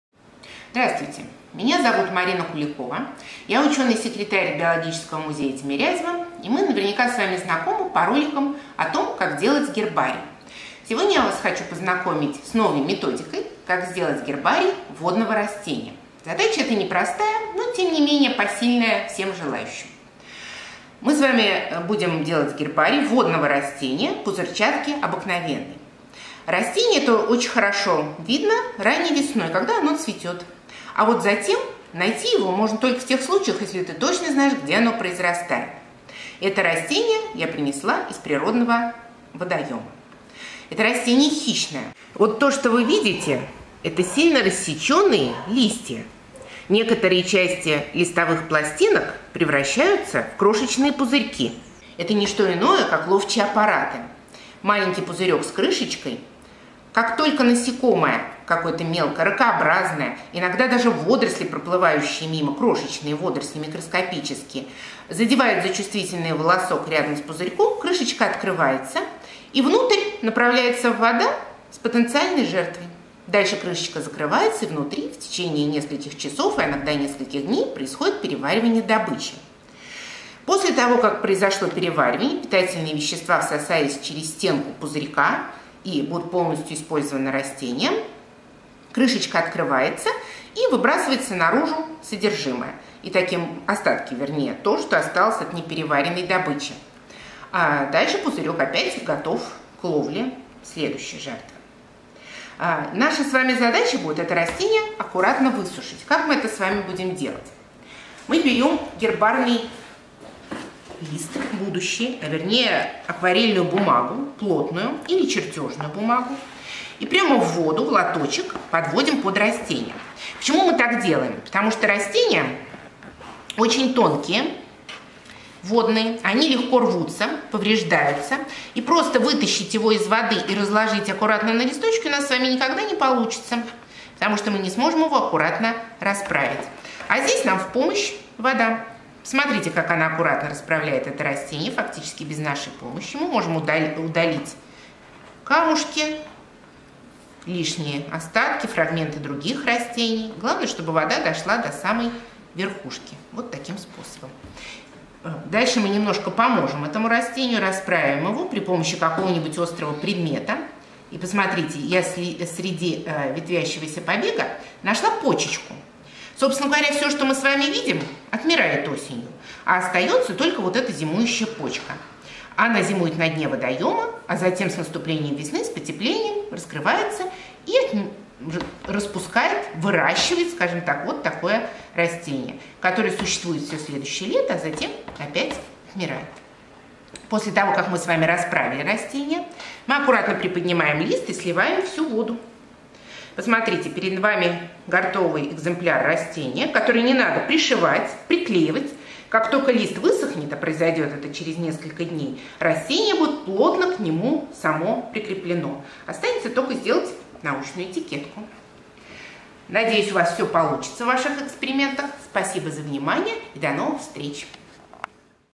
Пузырчатка
Ученый секретарь Биомузея Марина Куликова показывает, как делать из водного растения – пузырчатки.